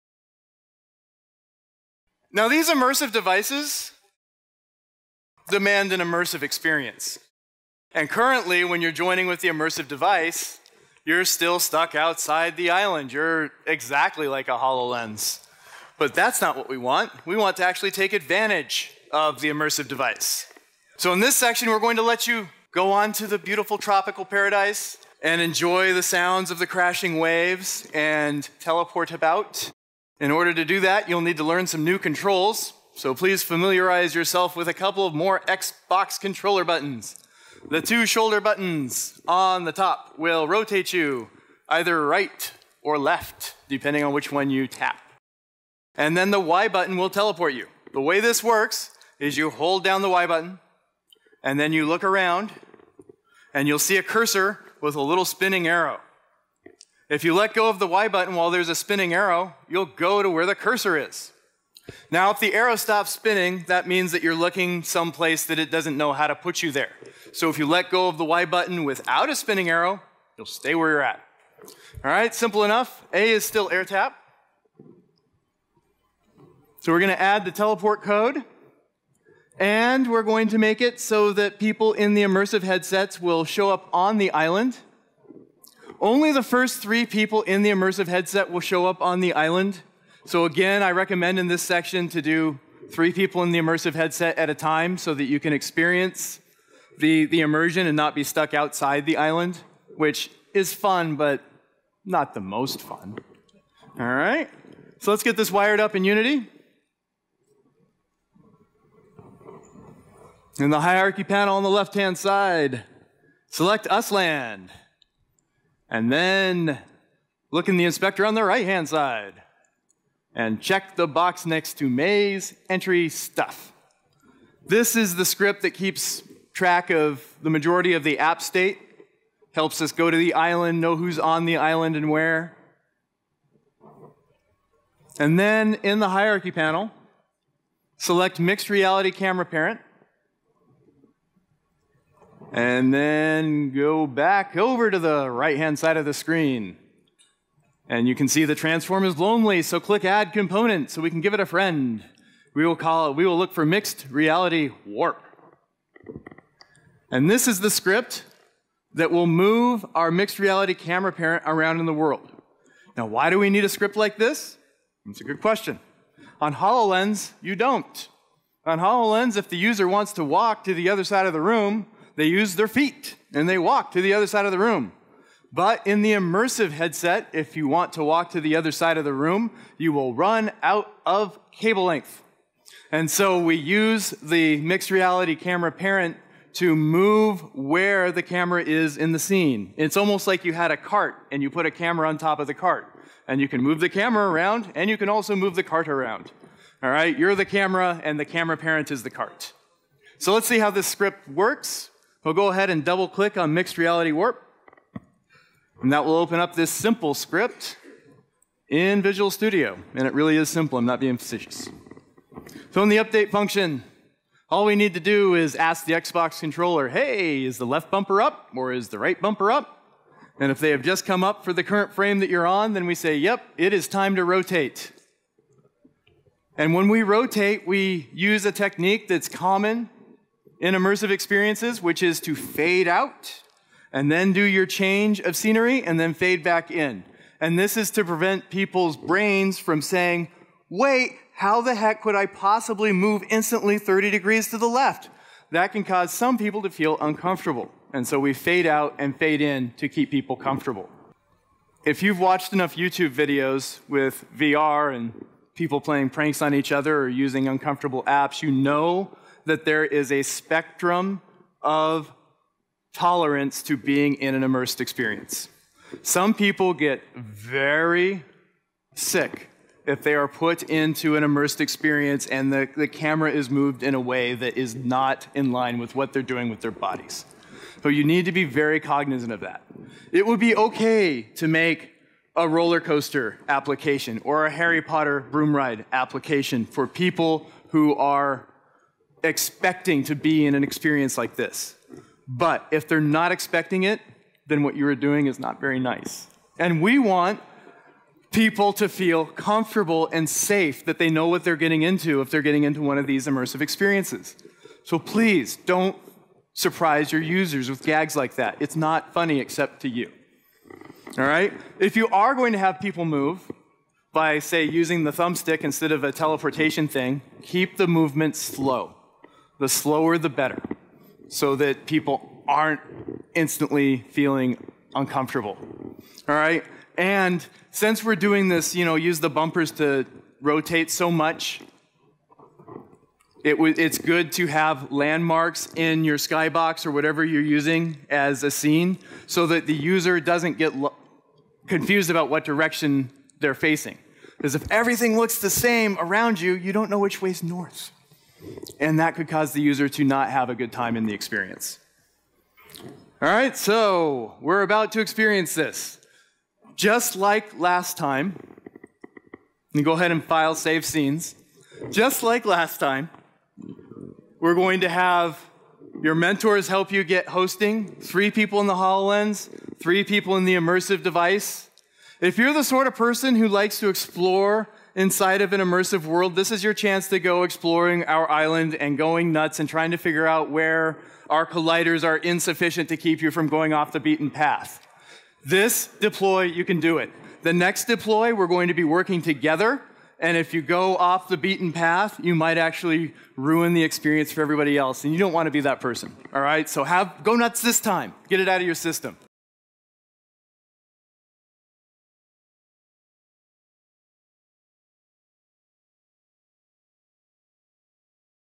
Microsoft HoloLens: Course 250 - Ch. 4. - Immersion and teleporting
This is the fourth chapter to the Mixed Reality Academy 250 course.  This series is composed of 5 chapters of a tutorial that will build a basic shared experience that runs on both HoloLens and Windows Mixed Reality immersive headsets.